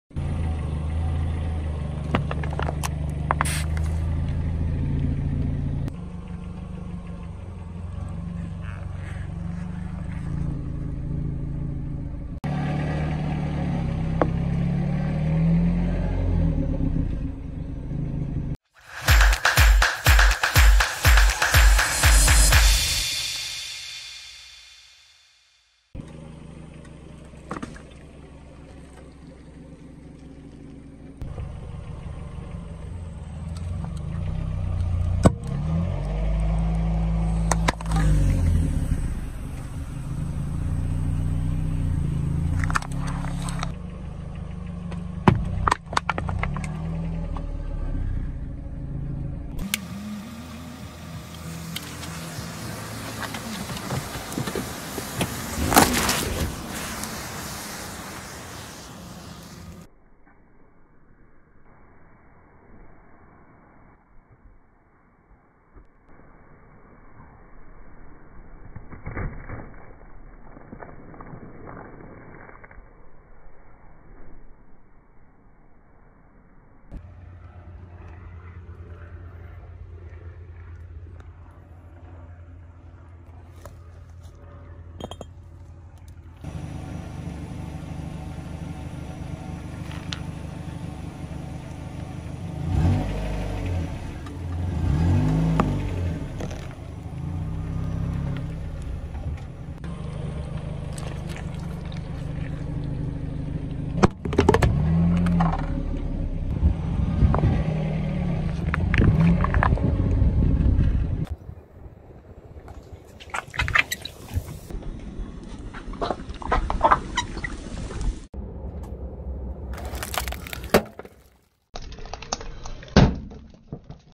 How destroy objects with a car | Diamond vs wheel | Top Wheel Destroying Objects videos
Don't try this at home! This is an experiment, a dangerous test with car.

N'essayez pas ca a la maison! C'est une expérience, un test dangereux avec la voiture.